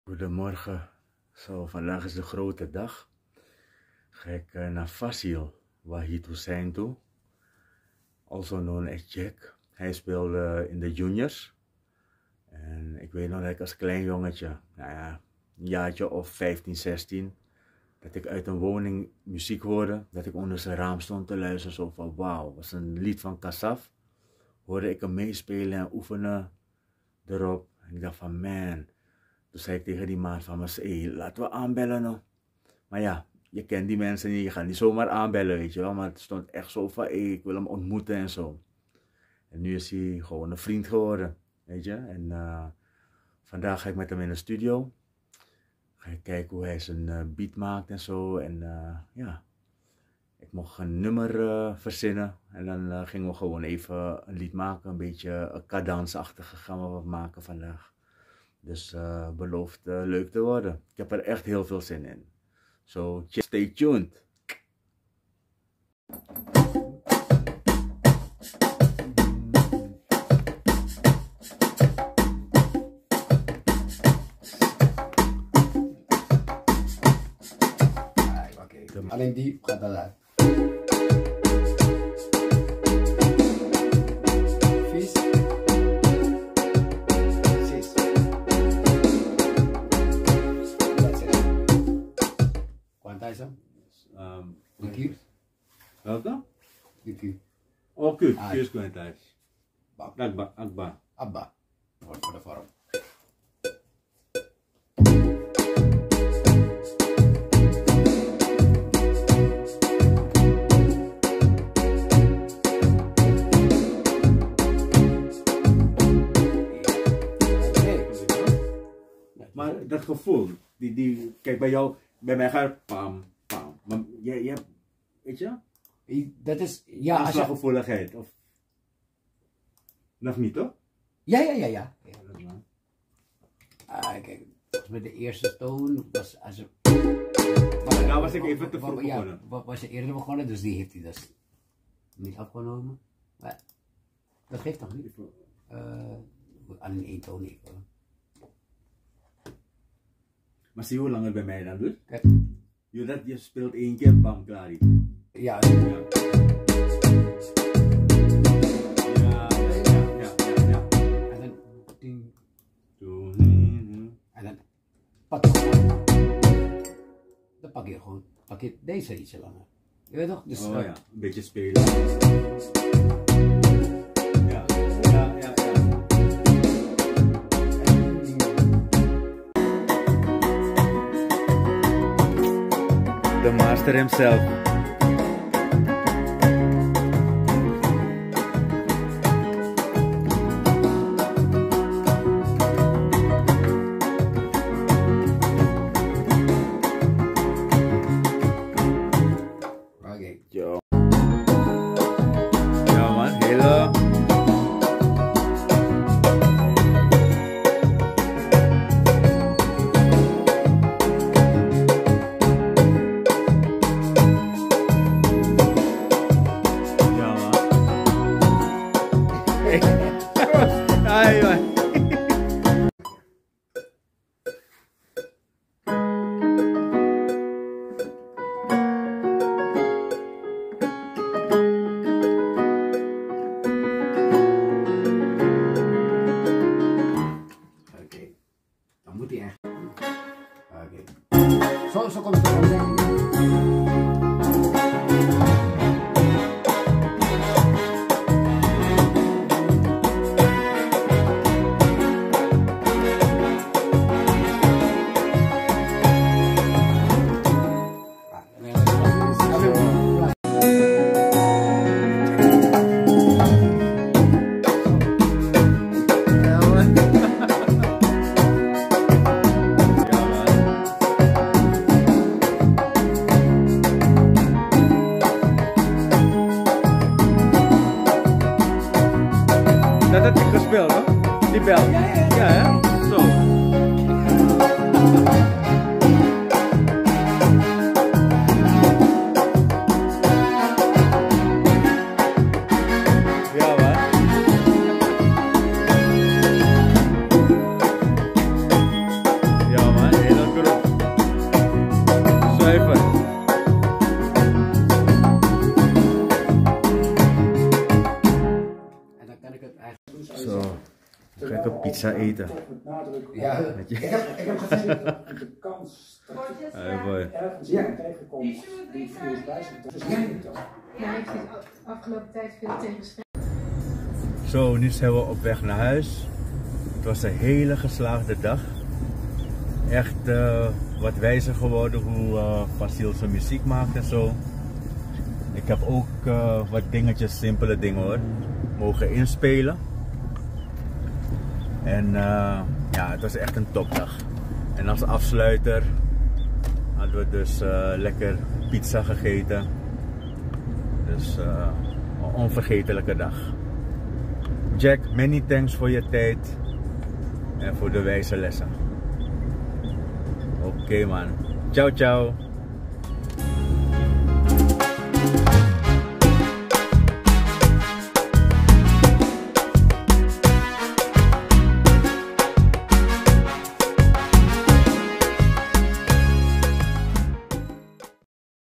In de studio met Faziel Wagid hosain🎹🎼🎵🎶🎹 vlog # 26
Ik neem een kijkje in de studio bij Faziel Wagid Hosain beter bekend als Jack. Hij is een bekende toetsenist en zanger in de Hindoestaanse en Surinaamse cultuur. Ook produceert hij muziek voor artiesten uit India, Suriname en Nederland.

Ik had het genoegen om samen met Jack de studio in te duiken. Ik mocht verzinnen wat voor nummer en ik koos ervoor om een Kizomba/ Kadans achtig nummer te maken.